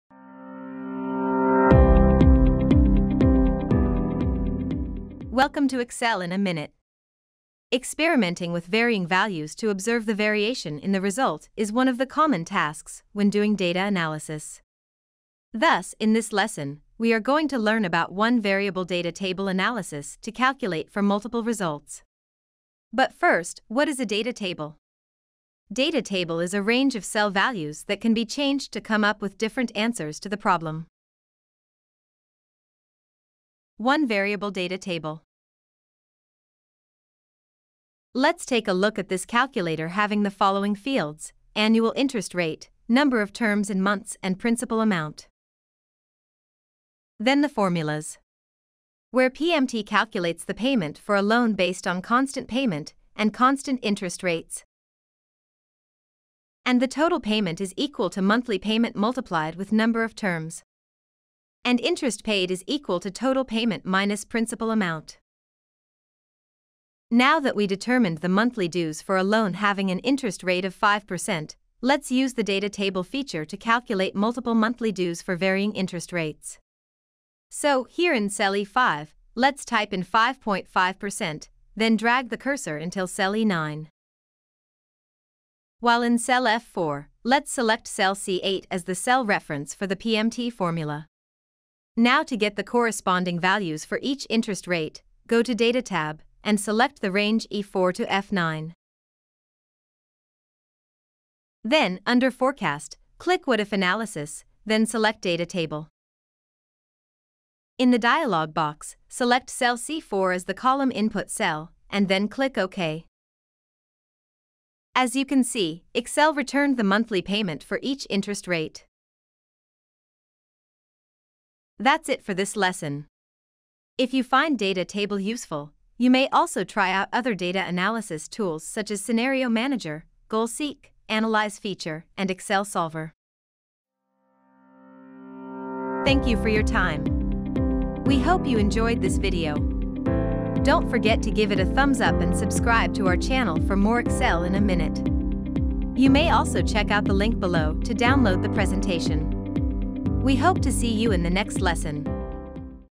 CALCULATE MULTIPLE RESULTS WITH DATA TABLE BY EXCEL IN A MINUTE
🤔 ABOUT THIS Lesson 👇
Watch this video to learn how to calculate multiple results using the Data Table tool!

Part I – Introduction (What is this lesson all about?)
Experimenting with varying values to observe the variation in the result is one of the common tasks when doing data analysis.

Part II – Body (When and where can the lesson be used?)
Thus, in this lesson, we are going to learn about one variable data table analysis to calculate for multiple results.

But first, what is a data table?

Data table is a range of cell values that can be changed to come up with different answers to the problem.

to download the files go to the bottom of the BLOG POST and press the download button